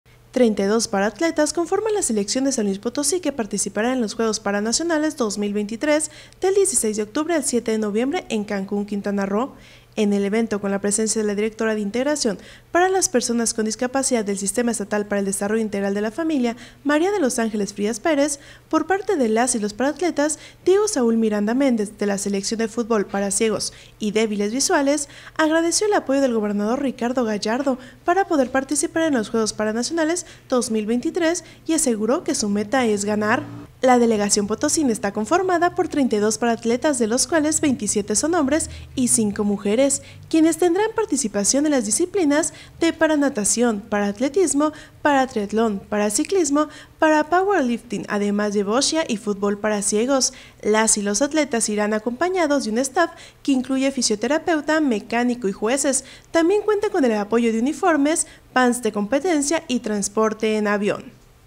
32 paraatletas potosinos partieron a los Juegos Paranacionales 2023 en Cancún
32 paraatletas potosinos partieron a los Juegos Paranacionales 2023 en Cancún